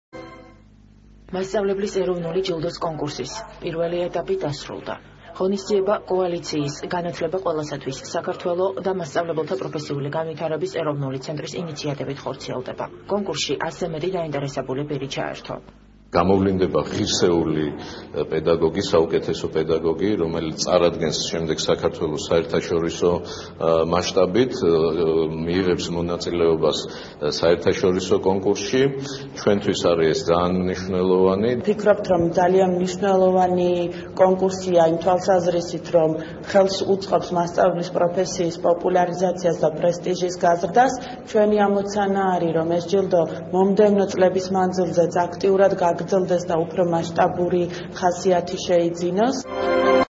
19 07 2017 ერთსულოვნება სიახლენი 19 00 / სიახლენი 22 00
კოალიციის „განათლება ყველასათვის - საქართველო“ და მასწავლებელთა პროფესიული განვითარების ეროვნული ცენტრის ინიციატივითა და ორგანიზებით მიმდინარე „მასწავლებლის ეროვნული ჯილდოს კონკურსის პირველი ეტაპი დასრულდა. საგანგებოდ კამპანიისათვის შექმნილ ვებგვერდზე 745 დაინტერესებული პირი დარეგისტრირდა. მათგან 114-მა მასწავლებელმა შემოიტანა განაცხადი.

პრეტენდენტების საერთო რაოდენობიდან 104 ქალბატონია, ხოლო 10 - მამაკაცი. მათგან საზ. მეცნიერებების მიმართულების მასწავლებელია - 20; უცხოური ენების - 11; ინფორმაციულ-საკომუნიკაციო ტექნოლოგიების - 1; სპორტის - 2; მათემატიკის - 8; სახელმწიფო ენების - 23; ესთეტიკური აღზრდის - 1; სხვადასხვა მიმართულებების - 29.

ტერიტორიული დაყოფის მიხედვით, ყველაზე მეტი - 37 განაცხადი შემოსულია თბილისიდან. პრეტენდენტების რაოდენობით მეორე ადგილზეა იმერეთის რეგიონი, 17 განაცხადით.

კონკურსის შემდეგ ეტაპზე სპეციალურად მასწავლებლის ეროვნული ჯილდოსთვის შექმნილი ჟიური განიხილავს შემოსულ განაცხადებს და შეარჩევს საუკეთესო ათეულს, ამის შემდეგ კი, პირისპირ ინტერვიუების საფუძველზე შეირჩევა ხუთეული, რომელთაგან ერთ-ერთი 5 ოქტომბერს, მასწავლებლის მსოფლიო დღეს, „მასწავლებლის ეროვნული ჯილდოს“ პირველი მფლობელი გახდება, გარდა ამისა, მას, ფულადი პრიზის სახით, კამპანიის გენერალური სპონსორისგან - ინა გუდავაძე-პატარკაციშვილისგან 10 000 ლარი გადაეცემა.

„მასწავლებლის ეროვნული ჯილდოს“, ასევე, მხარს უჭერენ განათლებისა და მეცნიერების სამინისტრო, ლიბერთი ბანკი, საზოგადოებრივი მაუწყებელი და ტელეკომპანია „იმედი“.

„მასწავლებლის ეროვნული ჯილდო“ კოალიცია „განათლება ყველასათვის - საქართველოს“, მასწავლებელთა პროფესიული განვითარების ეროვნული ცენტრსა და განათლებისა და მეცნიერების სამინისტროს შორის გაფორმებული მემორანდუმის საფუძველზე, 2017 წლის 13 მაისს დაწესდა. აღნიშნული ინიციატივა საქართველოში პირველად ხორციელდება და ის მასწავლებელთა პოპულარიზაციისკენ მიმართული გლობალური კამპანიის - „The Global Teacher Prize“ ნაწილია.

„მასწავლებლის ეროვნული ჯილდო“ მიზნად ისახავს წარმატებული მასწავლებლების გამოვლენას, საზოგადოებისთვის მათ წარდგენას და ამ გზით პროფესიის პრესტიჟის ზრდას.